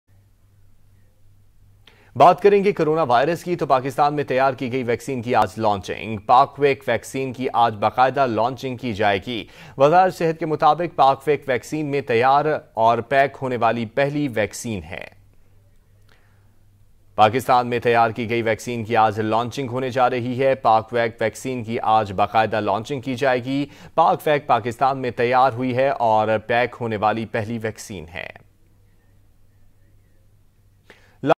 Vaccine Manufactured in Pakistan will be Launched Today | Such Tv
Vaccine Manufactured in Pakistan will be Launched Today..!!